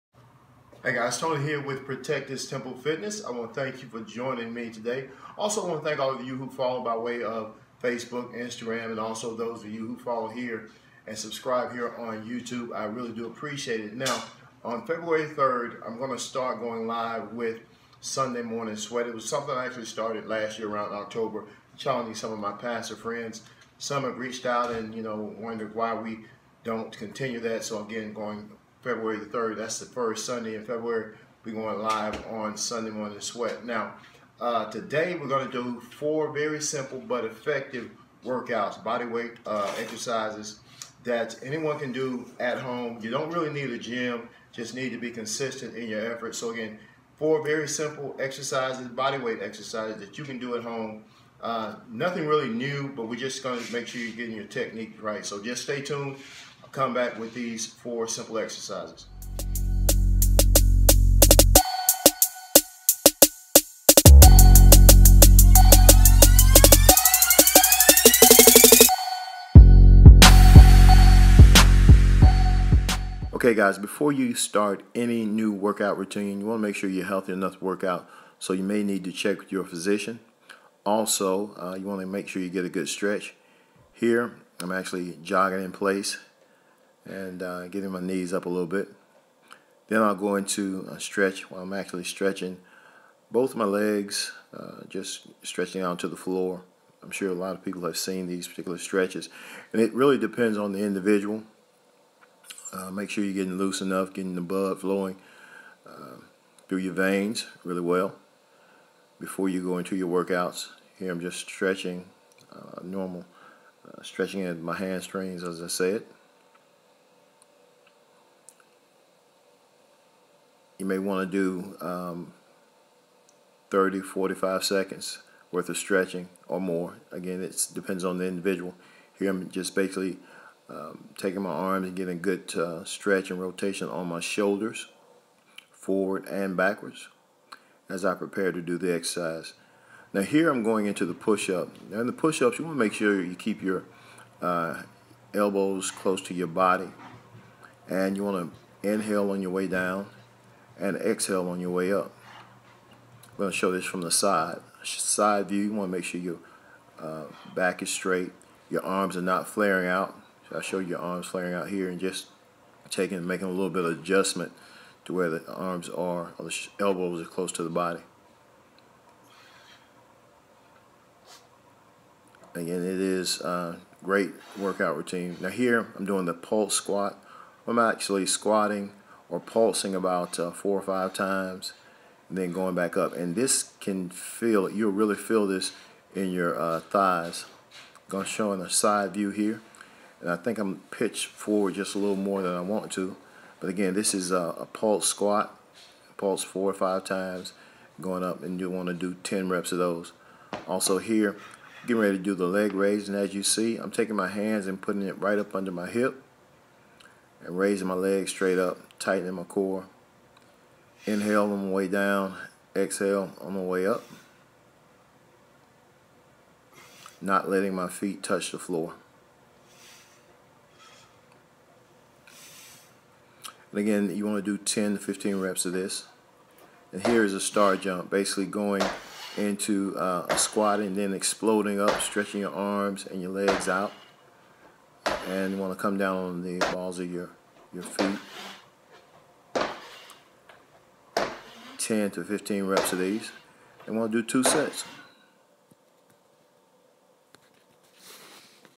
21 Day Workout-Day 11
This is the 11th day of my 21 day challenge. These are simple exercises almost YOU can do in the comfort of your home.

I’ll be adding more content soon as I continue to build my brand, Protect His Temple Fitness. Check me out on.